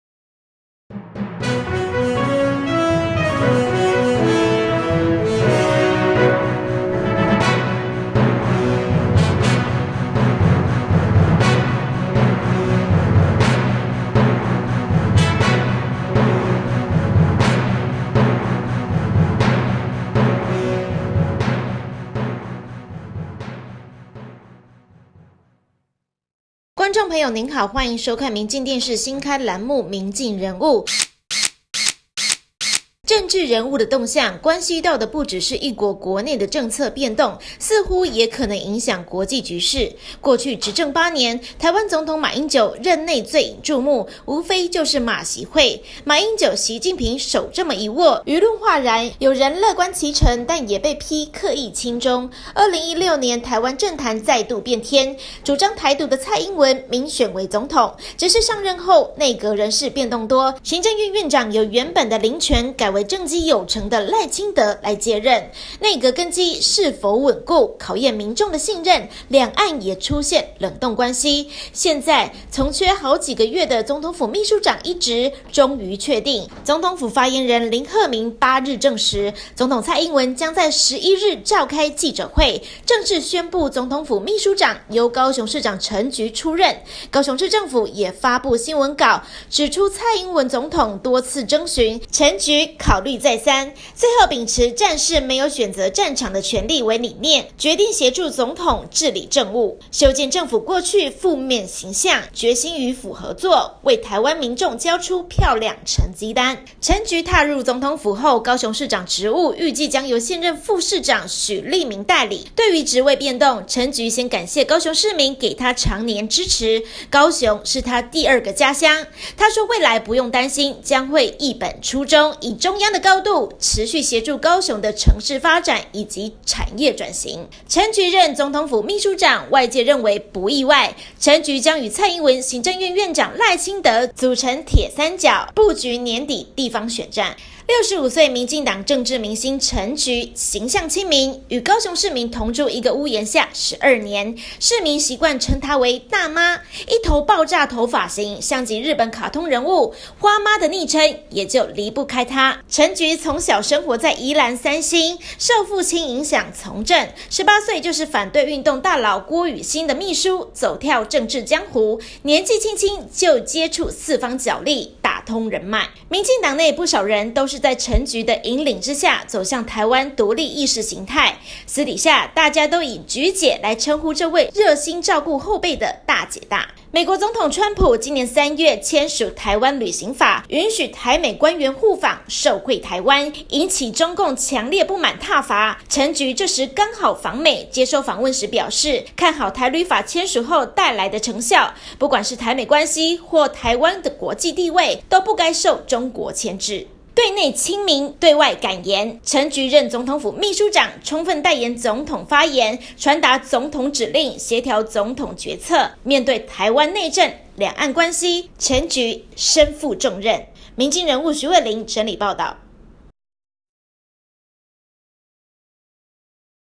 “菊姐”將出任台灣總統府秘書長，身負重責影響政治局勢（《明鏡人物》2018年4月9日）
明鏡人物：陳菊
政治人物的動向，關係到的不只是一國國內的政策變動，似乎也可能影響國際局勢，台灣從缺好幾個月的總統府秘書長一職，終於確定，總統府發言人林鶴明8日證實，總統蔡英文將在11日召開記者會，正式宣布總統府祕書長，由高雄市長陳菊出任，高雄市政府也發布新聞稿，指出總統蔡英文多次徵詢，陳菊考慮再三，最後秉持「戰士沒有選擇戰場的權利」的理念，決定協助總統治理政務。